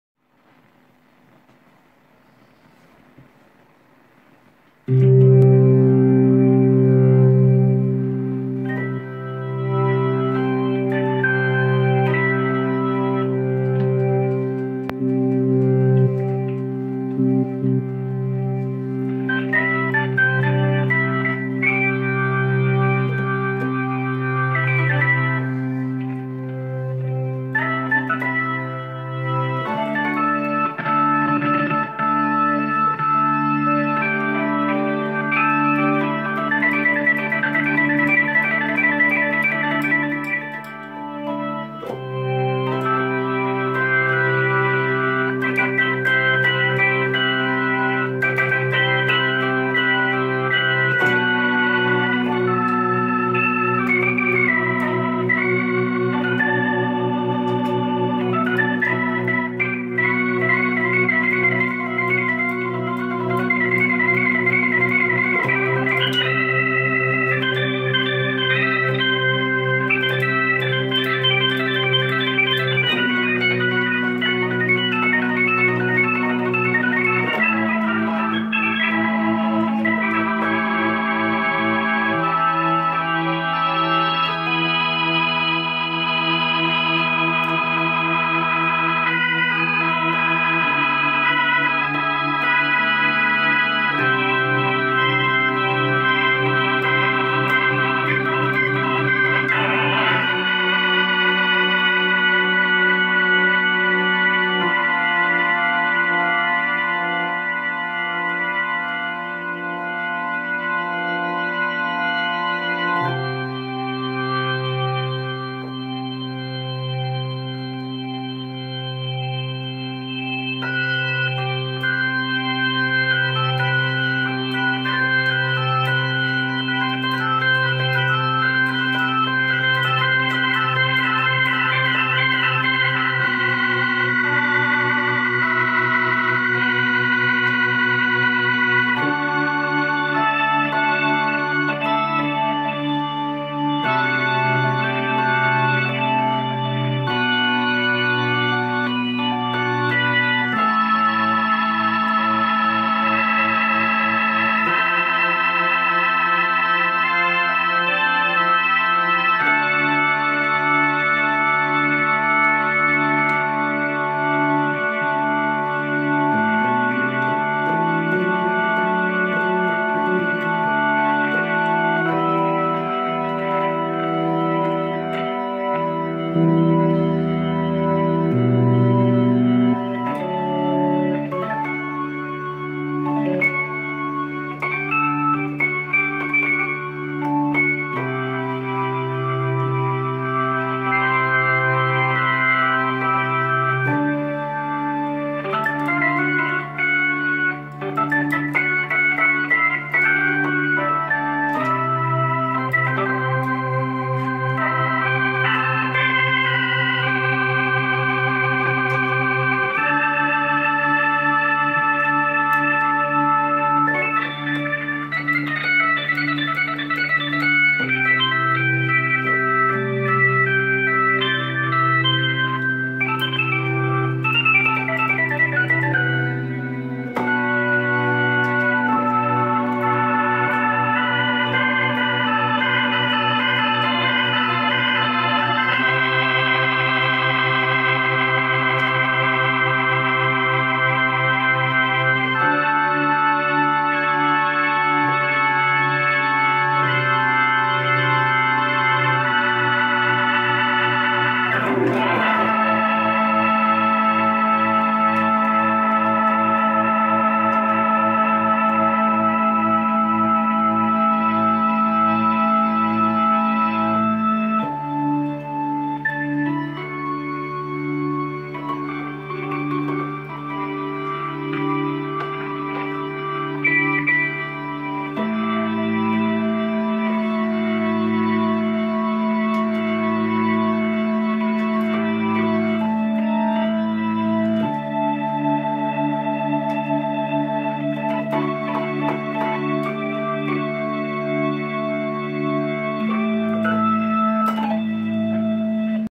Hammond X5 - Line 6 Roto
Into lovely little Roland Tubelogic 405, clean, farily flat.  First time I've; a) fired this up un ages, and b) played practically at all in about 9 months.  Ech.